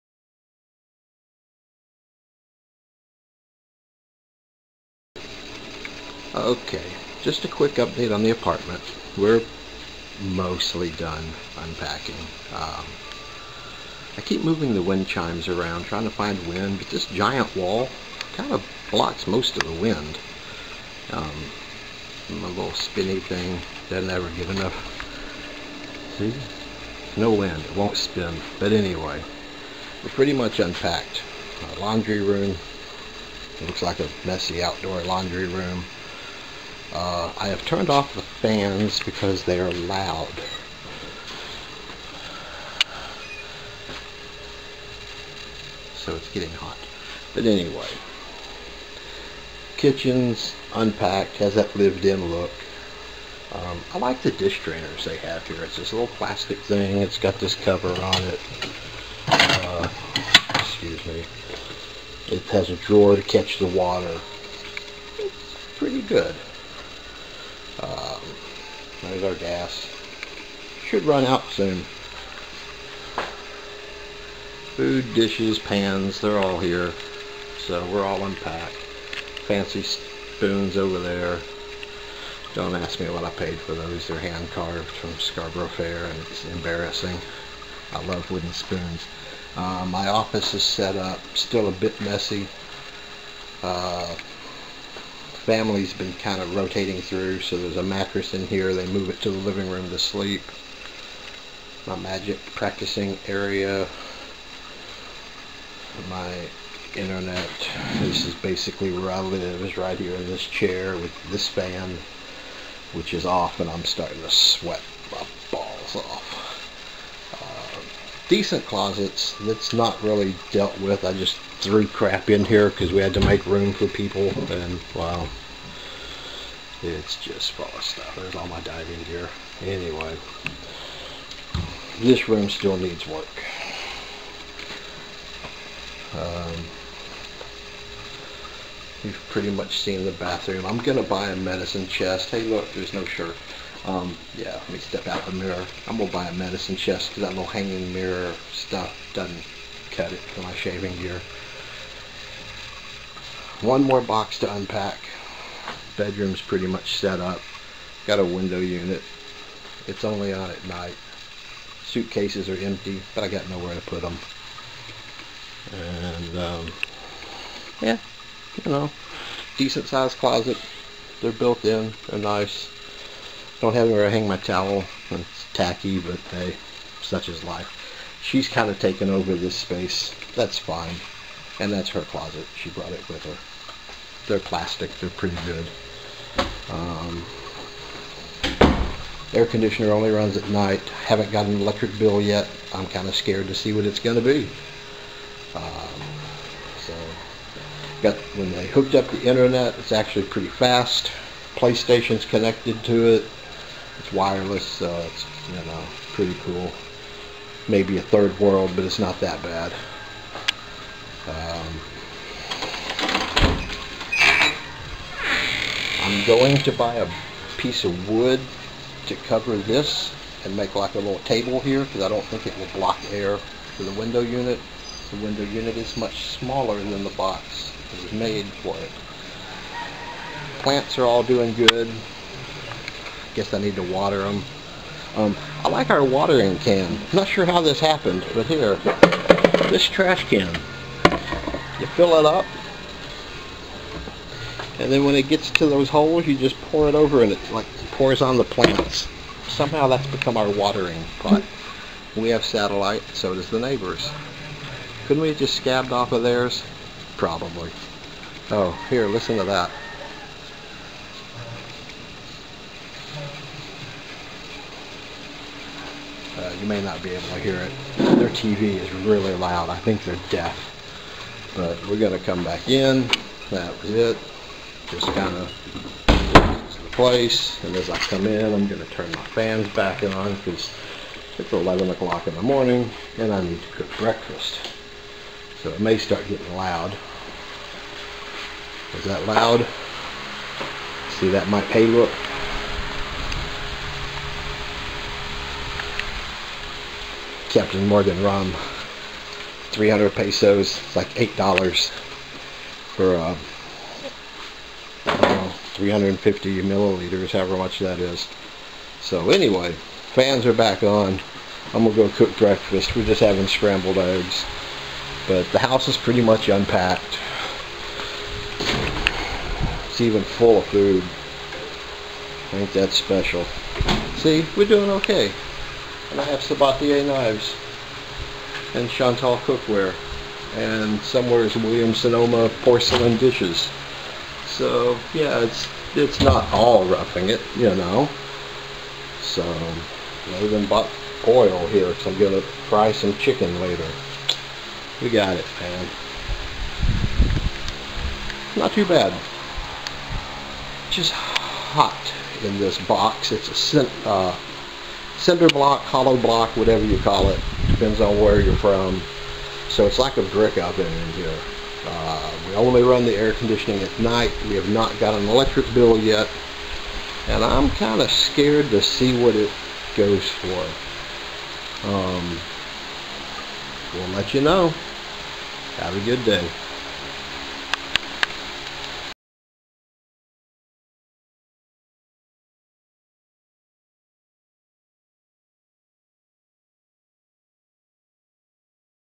Mandurriao, Iloilo Apartment Part 2
We got mostly unpacked.  Probably all we are going to do for a while.  We will finish in the rainy season... Just another look around the apartment.  Make sure and marvel at the home made coconut shell clapper and sail for wind chimes ha ha.